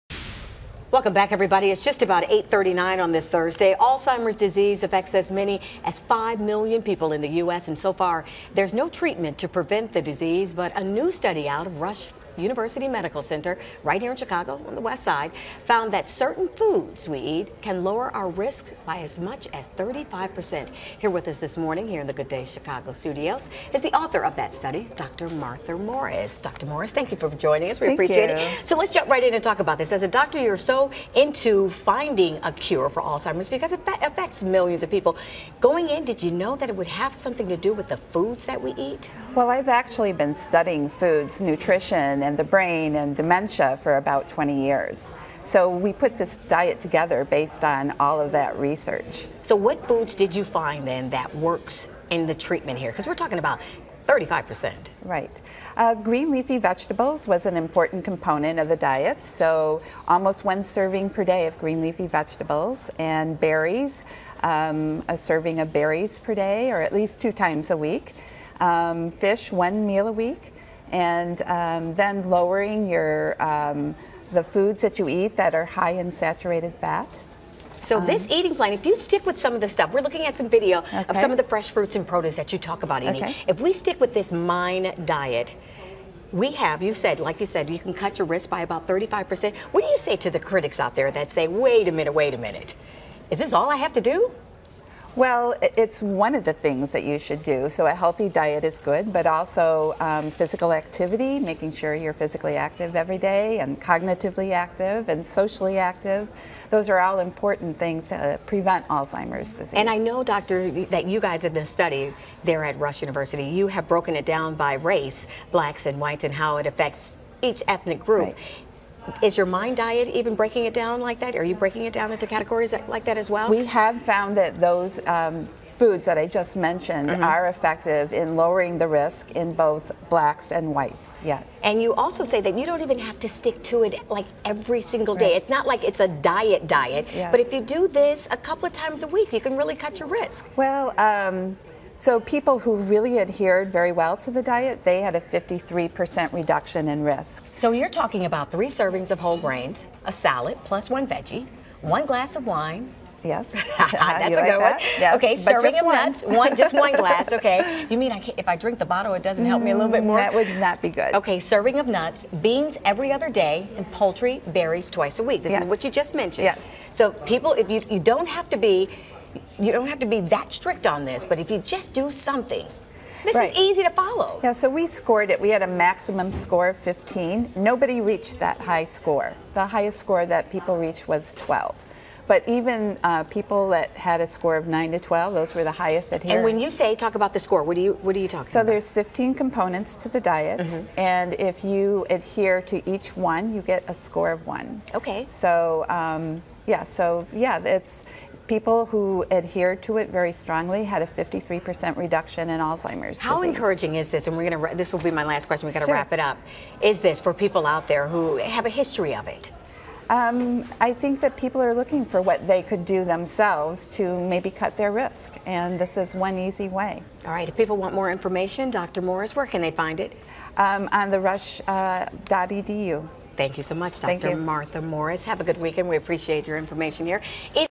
Dr Martha Morris 'The Mind Diet'
'The Mind Diet' reportedly slashes your risk of Alzheimer's Disease by 35 percent. Dr. Martha Morris of Rush University explains more.